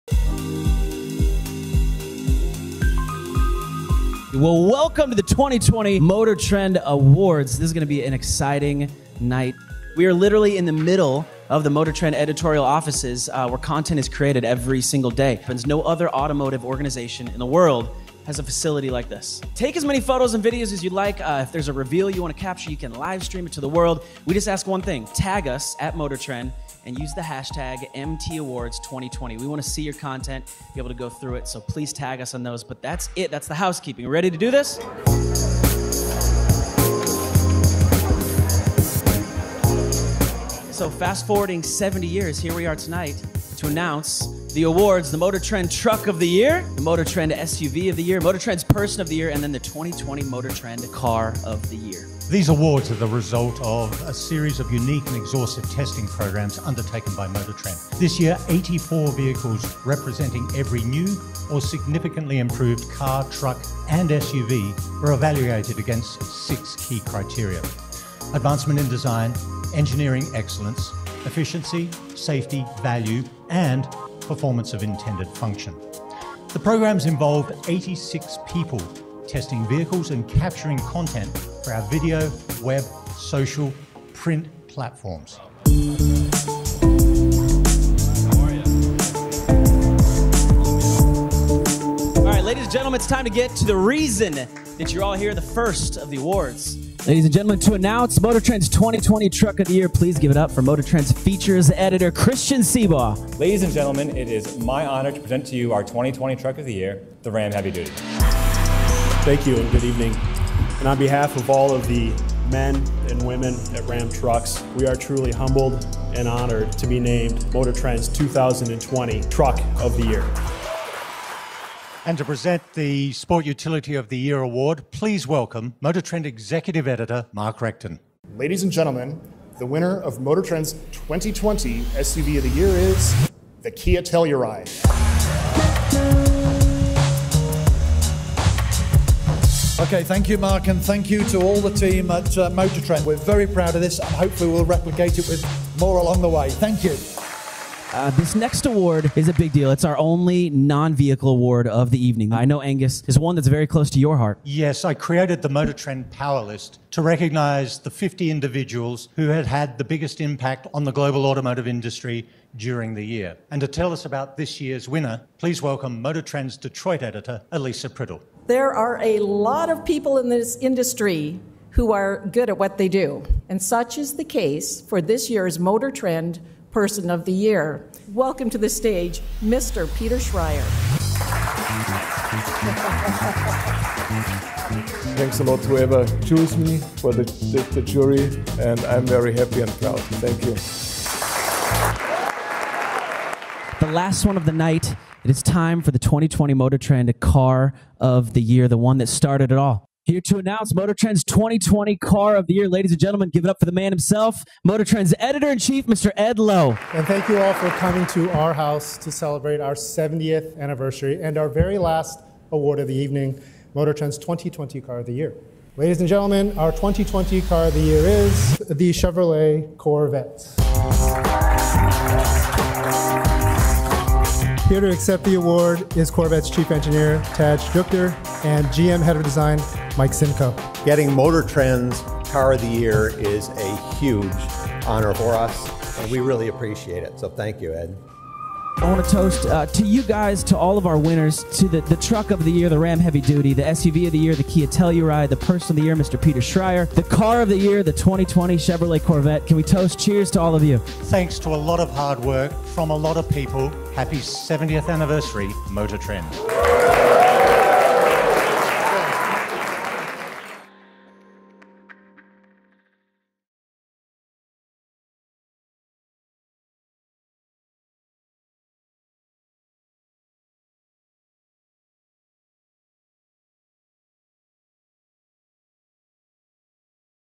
MotorTrend’s 2020 “Of the Year” Awards: Celebrating 70 Years of Journalism and Cars
For 70 years, MotorTrend has awarded its “Of the Year” Awards, most recently in four categories: Car, Truck, SUV and Person. After months of testing, driving, math, photos, videos and intense discussion, MotorTrend announced the winners at a glitzy event hosted right in our El Segundo headquarters. Check it out!